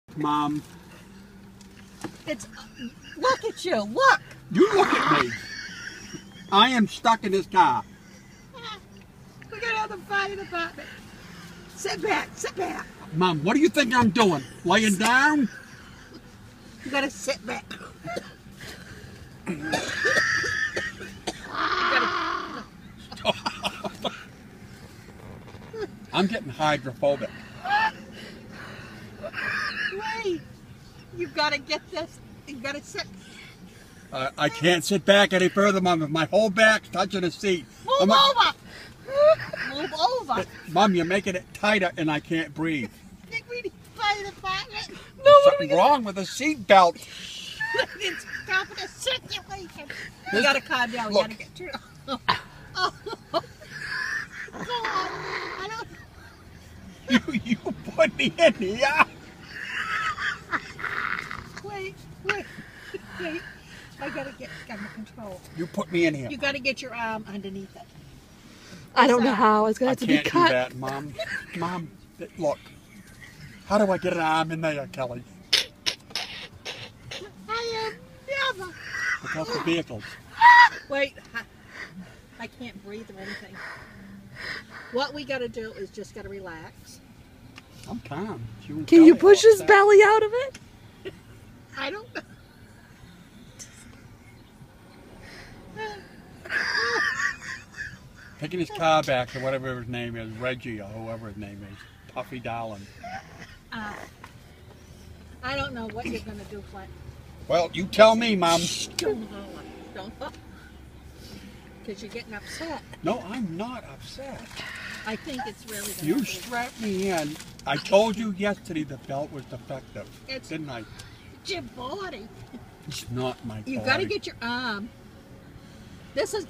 man stuck in seat belt
Pit stop to stretch legs and Clint gets stuck is seat belt, he does get out after we all start breathing and calm down.